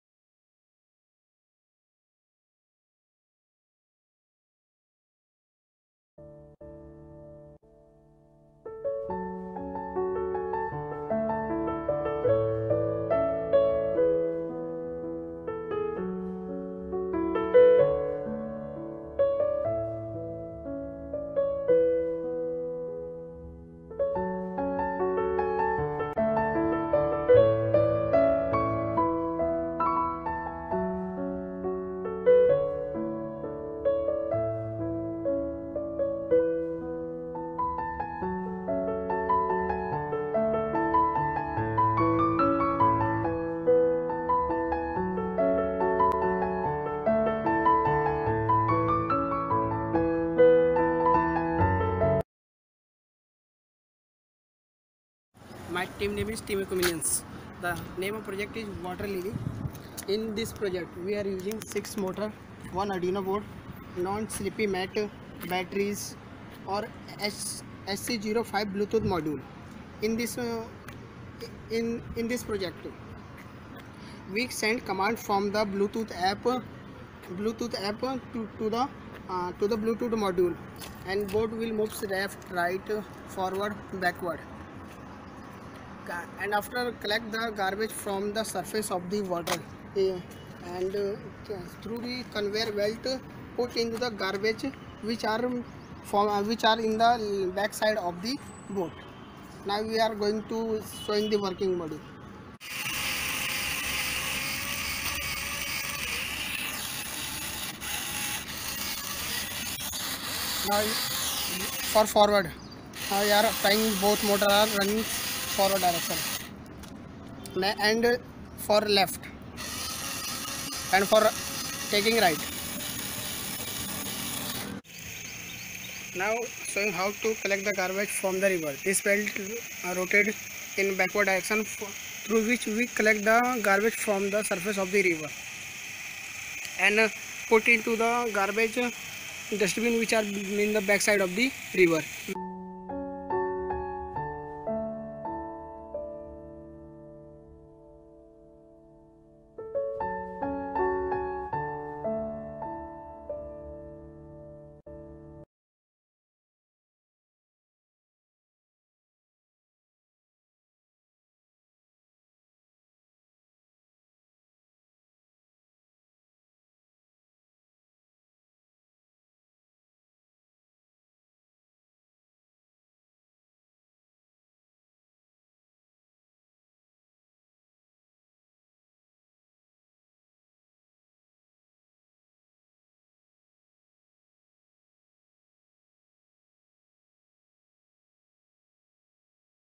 WaterLily {SIH-2019 Hardware Edition Official Entry for Round-2}
This is official submission for Round-2 evaluation of Smart India Hackathon-2019 Hardware Edition.

Project Name- WaterLily
Team Name- Eco Minions
College- Dronacharya Group of Institutions, Greater Noida